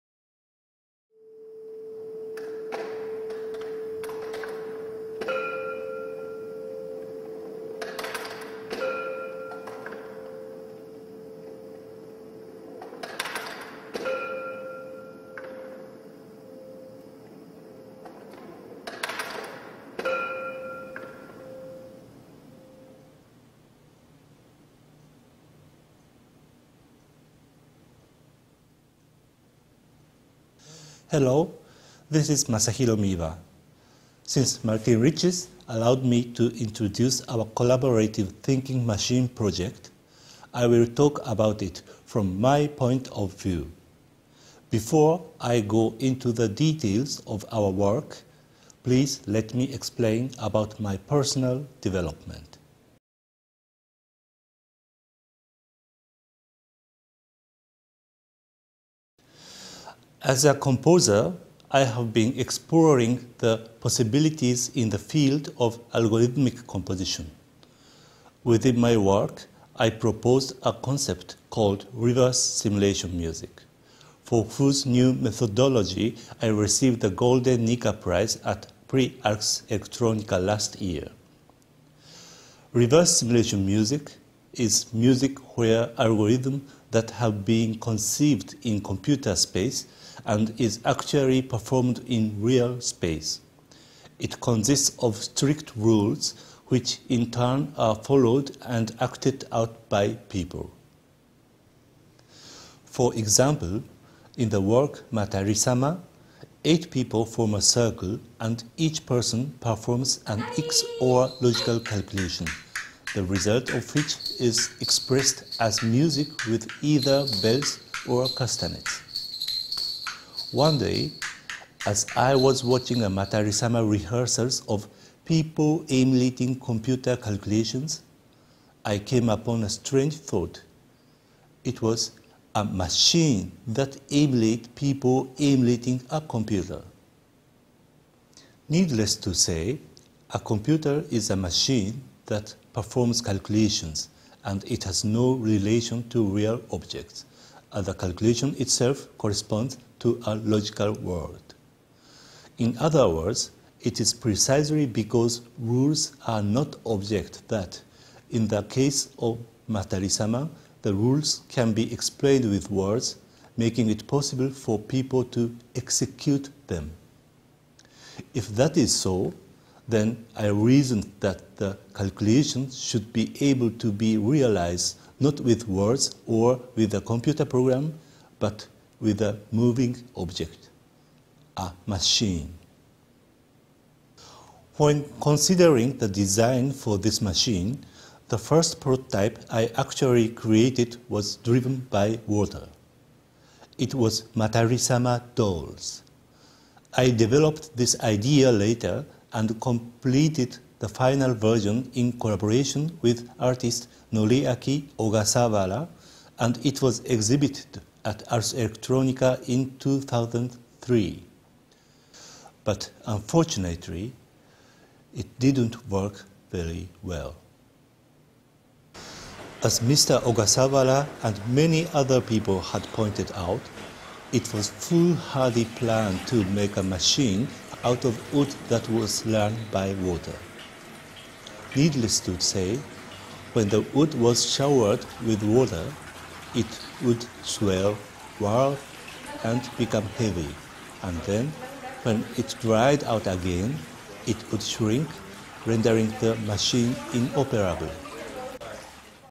The Thinking Machine part1/2
The Thinking Machine is a ternary computer that outputs melodies formed by three sounds as a result of the calculations. Whether binary of ternary, it is certainly a computer in Alan Turing's sense, although in this case the algorithmic mechanism is fixed so it is not a unversal Turing machine. However, like all computers - the Thinking Machine is a logical machine with input and output that can process and store code, and is an actual object that expresses discrete time within real time.